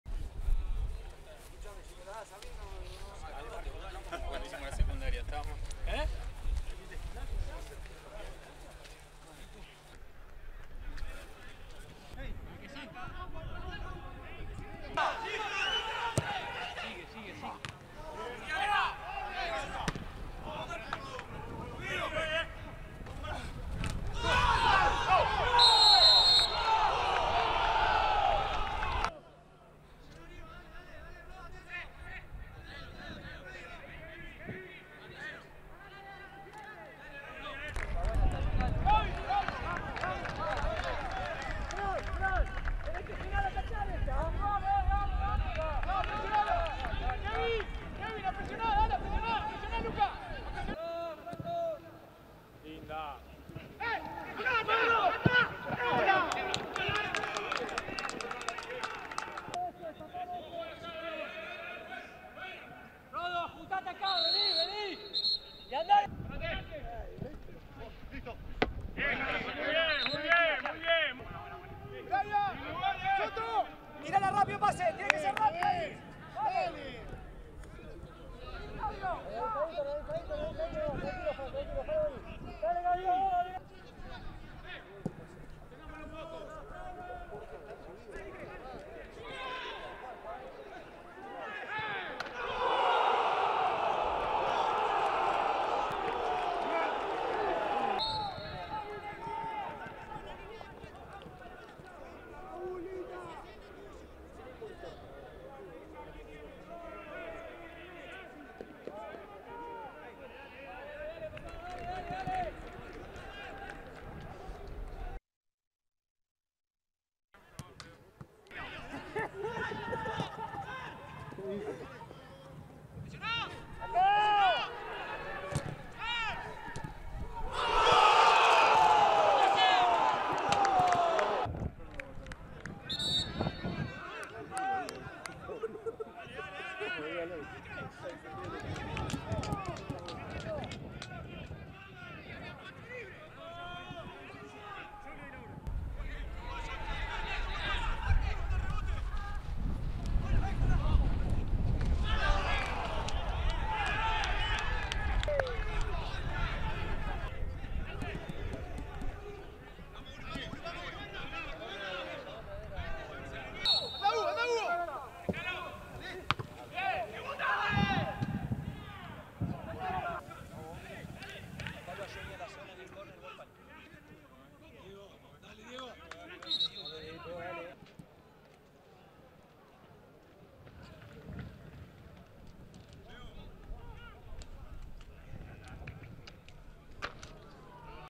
TorneoSocios | Fecha 1 | resumen de Huracán - Defensa y Justicia
Los tres puntos quedaron en el Ducó: con goles de Enrique Triverio y de Santiago Hezze, Huracán le ganó 2 a 1 a DefensayJusticia (Lucas Barrios) por la Fecha1 del TorneoSocios 🏆